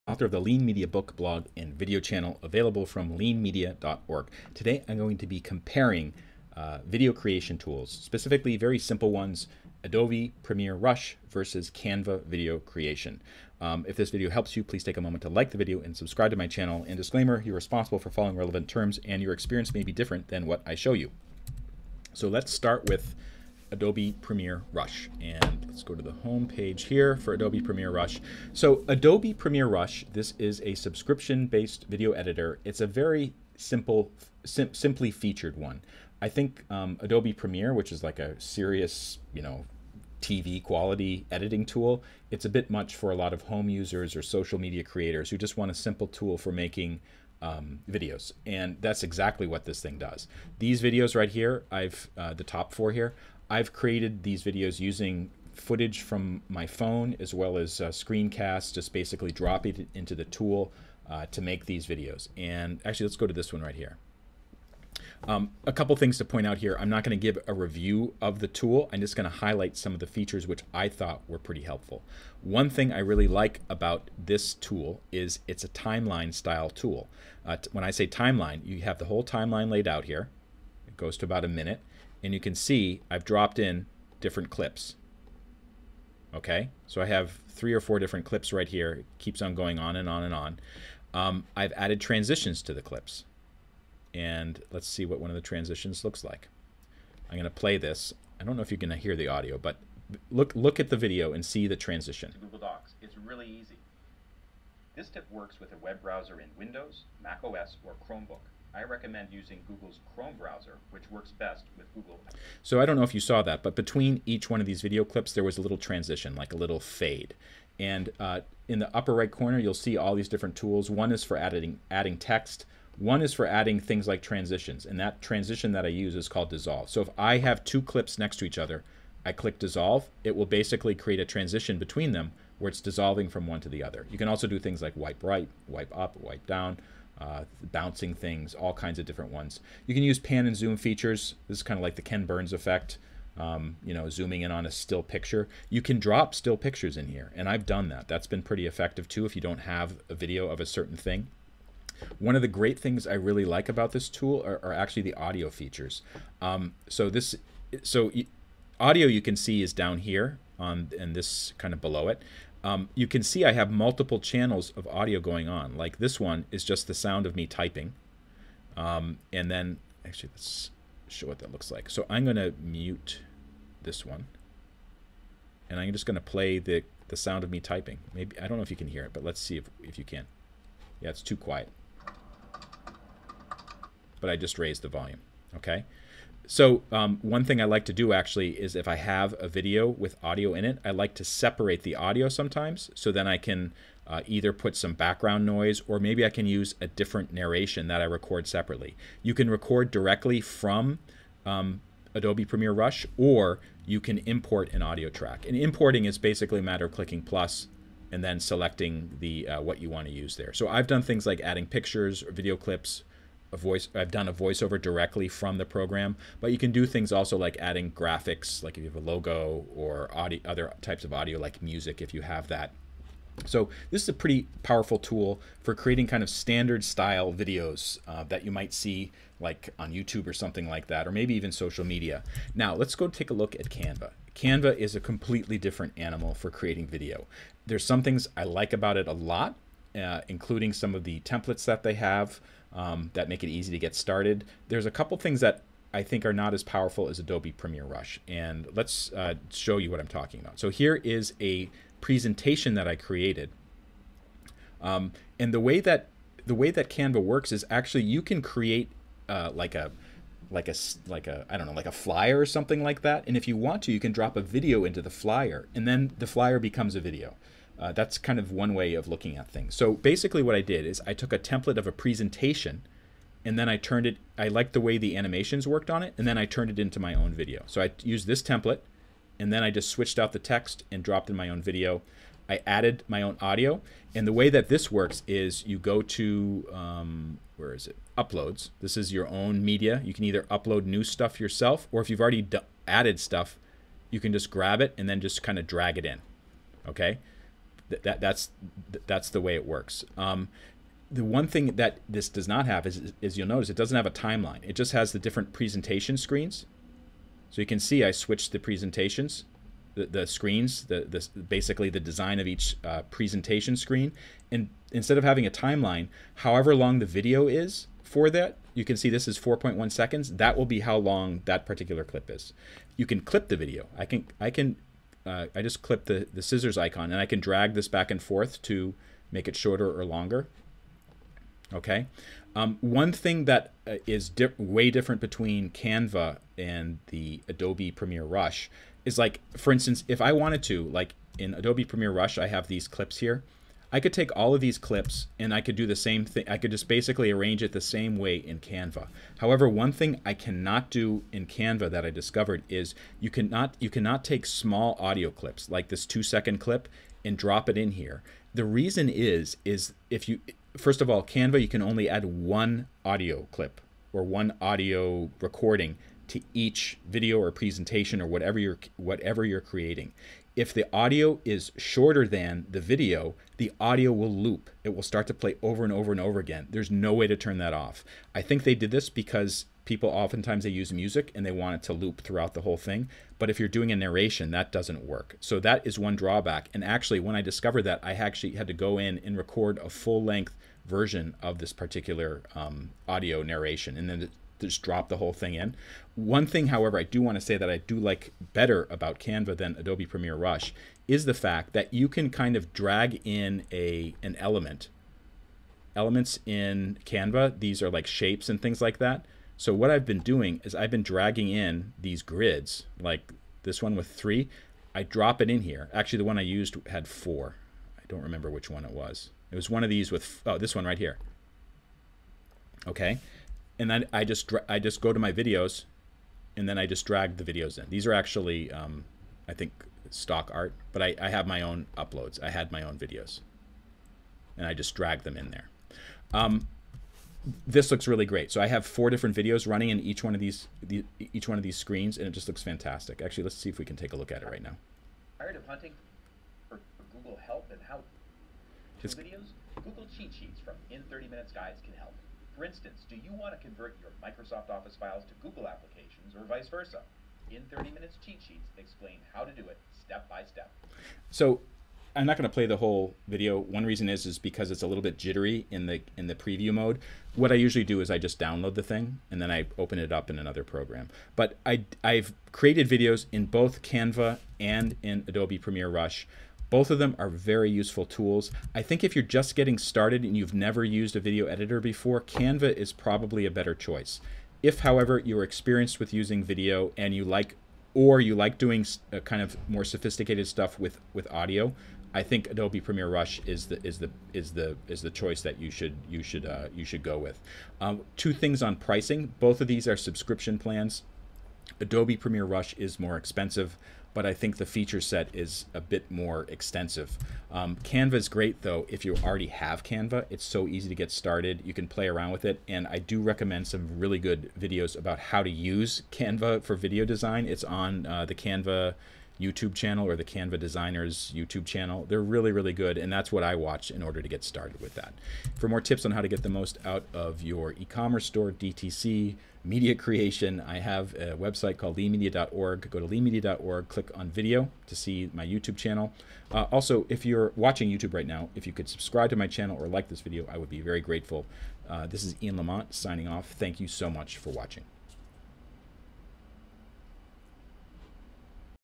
Video editor BAKE OFF: Adobe Premier Rush vs. Canva Video Editing
DESCRIPTION: An overview of how Adobe Premier Rush and Canva compare.